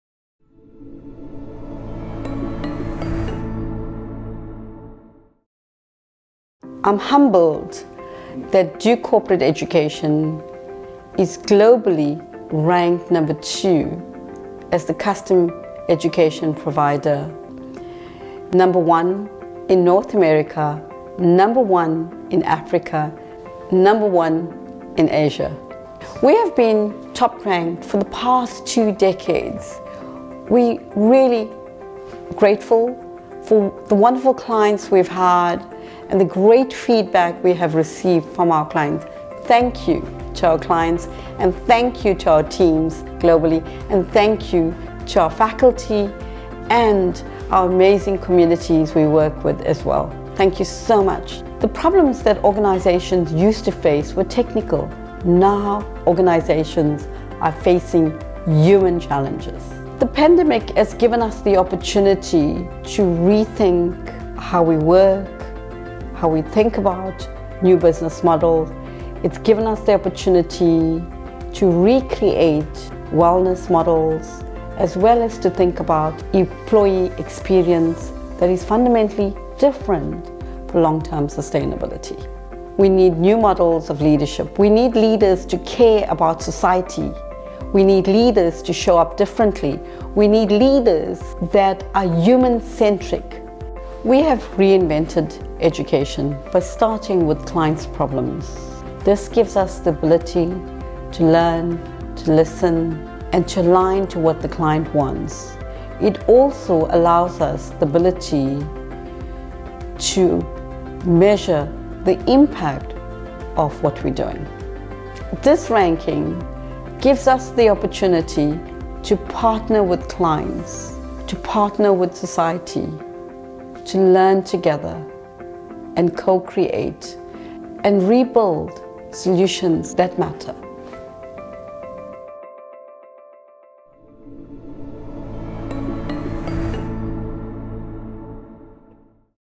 A message from CEO Sharmla Chetty on the Financial Times Rankings
Financial Times Custom Executive Education Rankings 2022

For over two decades, the @FinancialTimes has consistently ranked Duke Corporate Education (Duke CE) among the top custom education providers.

This year, Duke CE was #2 worldwide, and the top provider based in North America, Africa, Asia, and the UK.